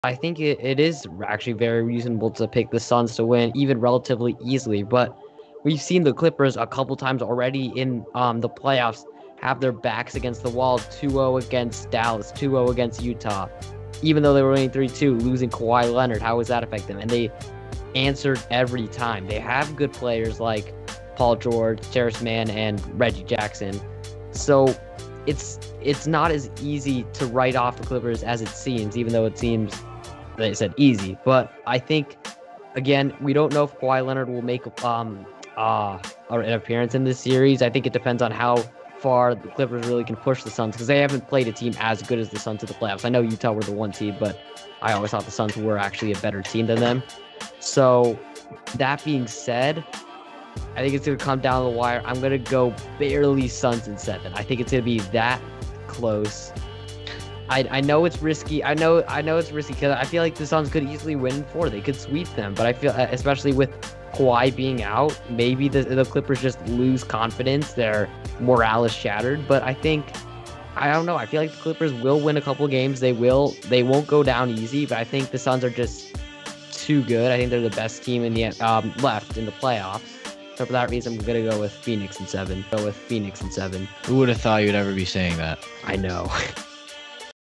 thisJUSTin SHORTS - Can the Clippers come back?
Can the Clippers come back against the Suns?

Suraj of Thee Sports Scoop is still picking Phoenix but isn't ruling out an LA rally.

See why he thinks this series will be closer than most people think.